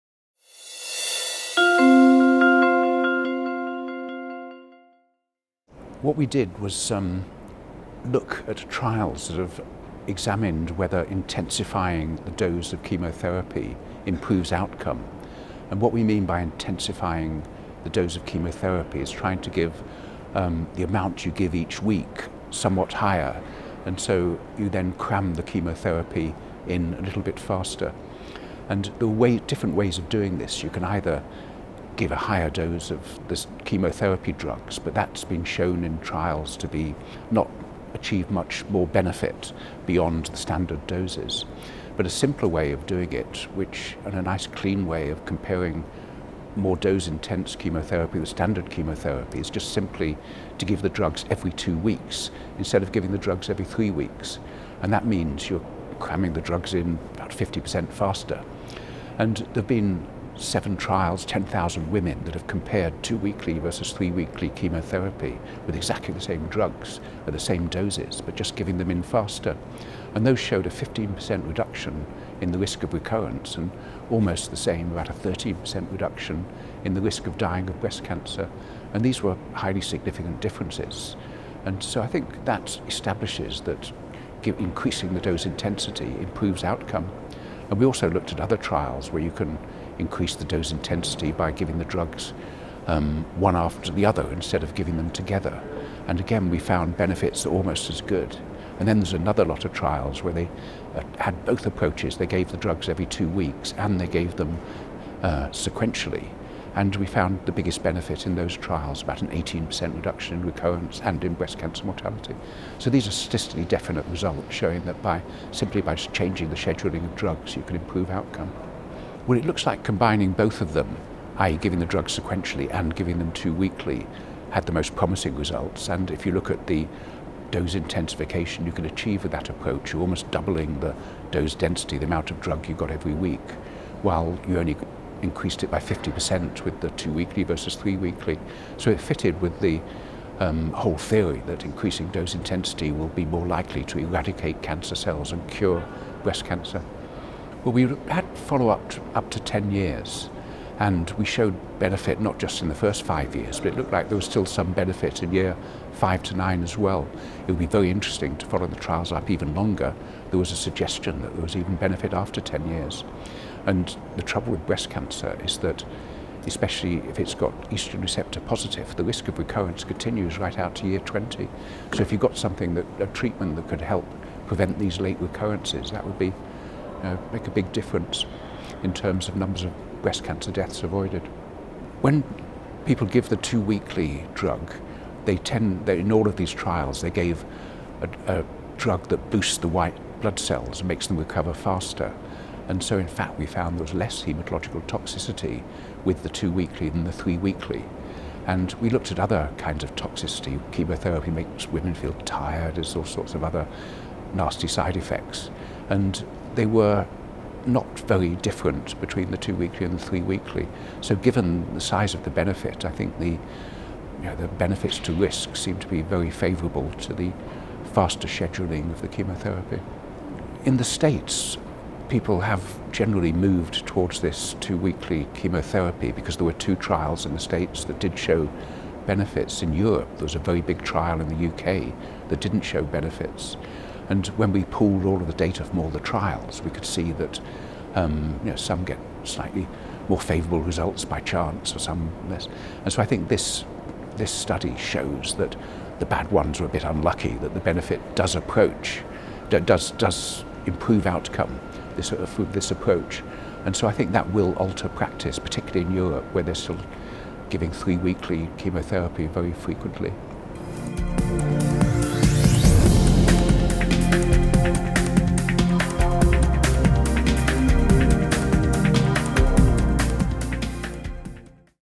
Increasing dose intensity of adjuvant chemo in breast cancer
Increasing the dose intensity of adjuvant chemotherapy reduced risks of breast cancer recurrence and death by about 15% in an Early Breast Cancer Trialists’ Collaborative Group (EBCTCG) meta-analysis of individual patient data, reported Dr. Richard Gray.